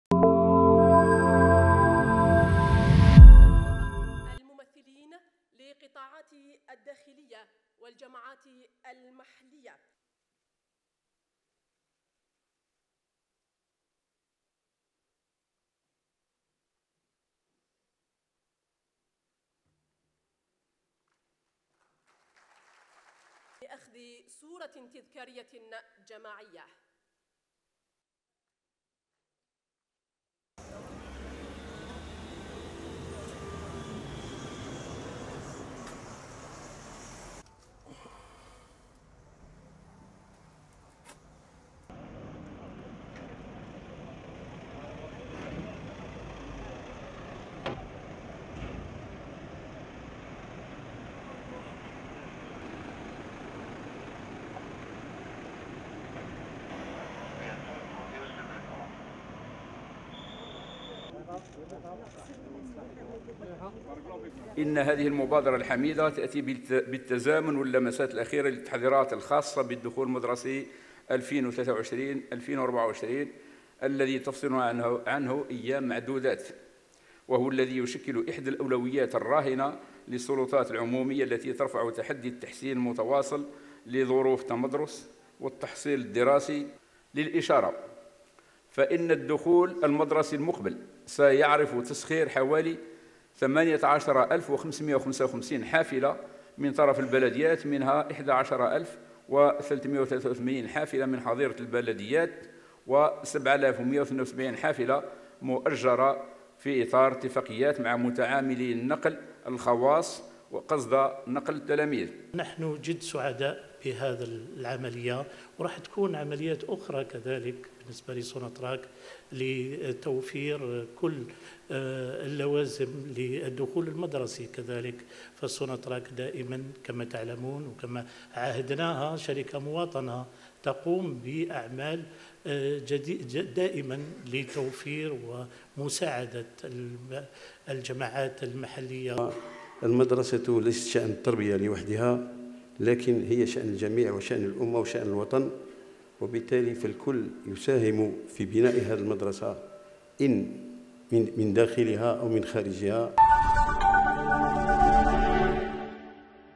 تسليم 60 حافلة نقل مدرسي من قبل مجمع سوناطراك لفائدة عدة بلديات عبر الوطن
الجزائر (وأج) - منح مجمع سوناطراك, اليوم الاثنين 60 حافلة نقل مدرسي لفائدة التلاميذ المتمدرسين على مستوى المؤسسات التربوية الواقعة بمناطق معزولة منتشرة عبر 60 بلدية تابعة ل 41 ولاية.